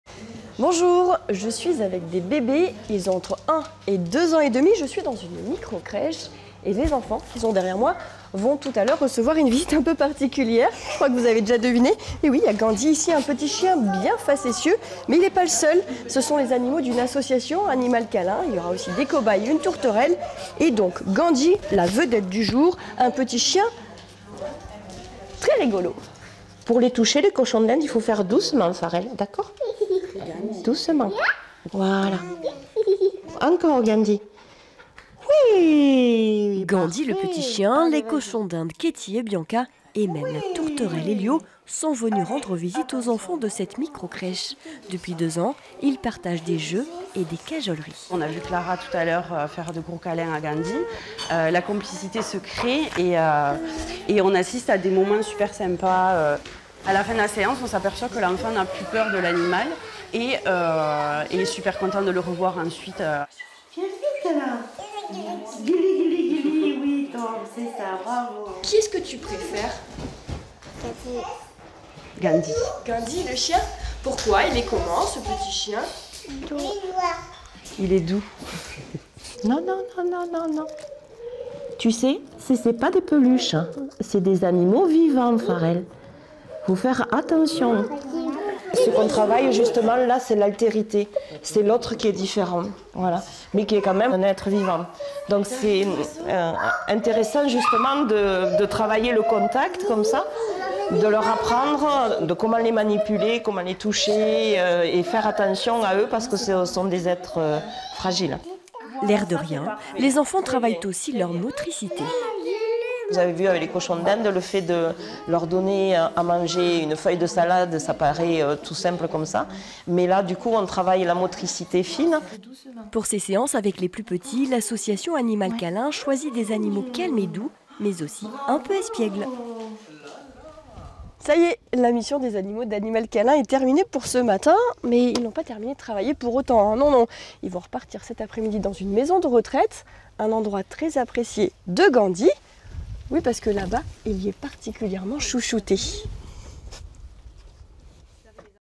Albi : chiens et perruches en visite à la crèche pour une séance de médiation animale
L'expérience menée à Albi est insolite mais surtout instructive. L'association tarnaise Animal Câlin propose aux plus jeunes d'approcher les animaux pour travailler leur rapport aux autre et leur motricité.

Miryam Brisse et Véronique Galy